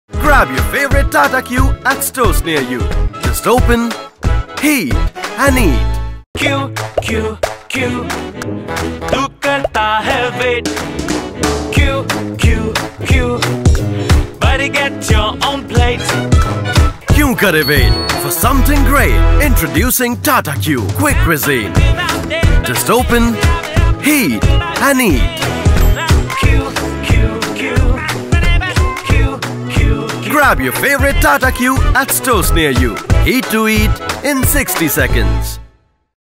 Tata Q Quick Quisine | Just Open, Heat & Eat
Why wait for something great?

Tata Q Quick Quisine’s delicious meals fulfil your cravings in just 60 seconds!

All you need to do is Open, Heat, and Eat!